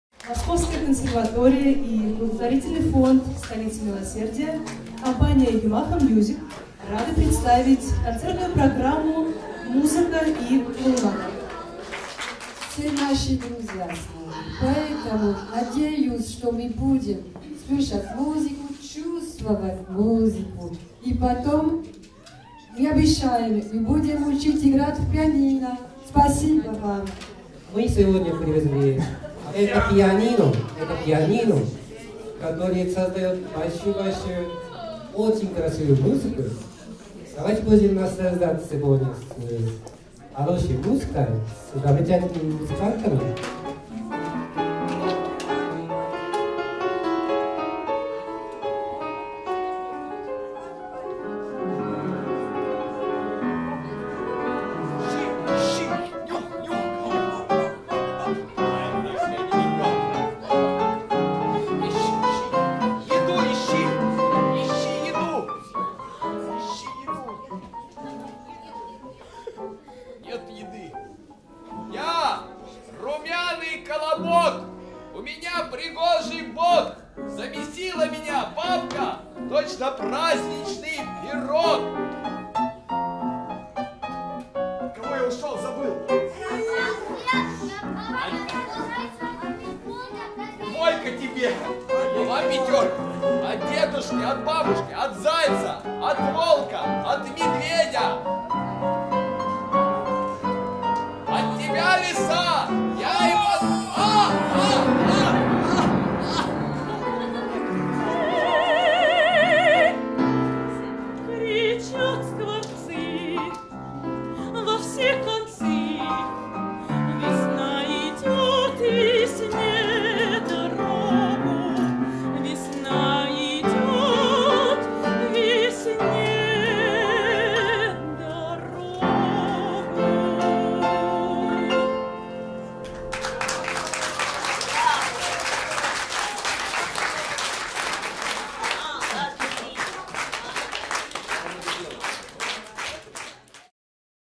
Консерватория — детям: «Музыка и клоунада»
Концерт в рамках Благотворительной программы «Консерватория — детям»
«Музыка и клоунада».
Олег Билик, Илья Боязный (клоунада),
Ольга Иванова (фортепианные импровизации),
Алена Розкопа (сопрано).
Ведущая — Марта Глазкова.